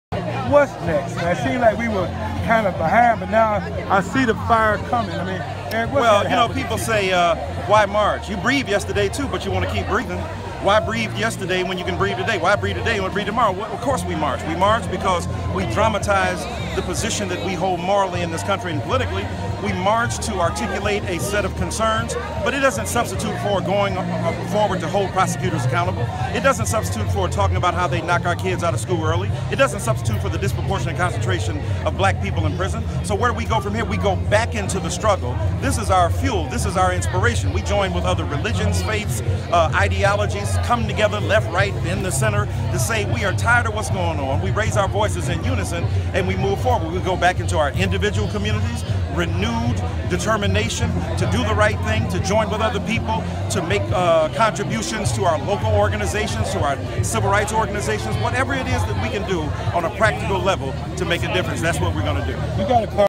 Michael Eric Dyson
Talks about "What's Next" after Al Sharpton's One Thousand Ministers for Justice March on August 28th.